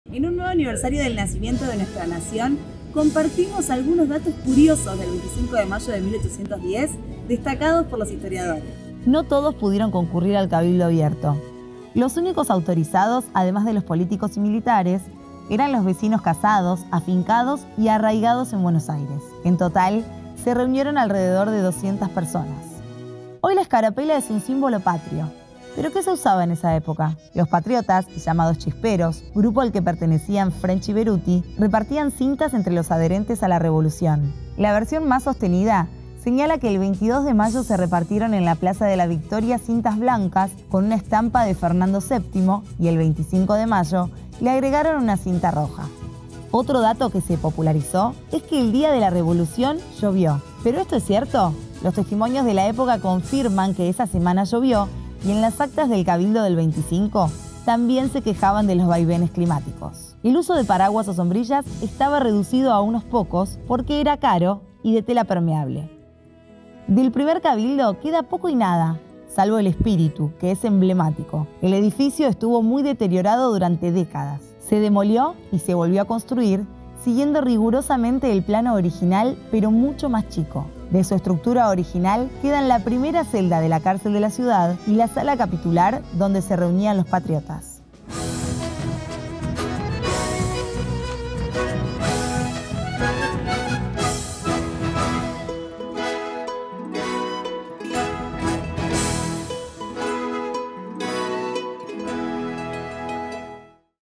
25 de mayo, curiosidades.
¡¡Viva la Patria!! 🇦🇷
📆 Este 25 de mayo te acercamos algunas perlitas de la revolución ✍🏻 Datos que no te enseñan pero que a los historiadores (chismosos) 👀 les gusta compartir ☔ 🥟🧉

¡Valoremos nuestra independencia, celebremos a nuestra nación! 💪🏻

¿Conocés alguna otra curiosidad de la época? Contanos 👇🏻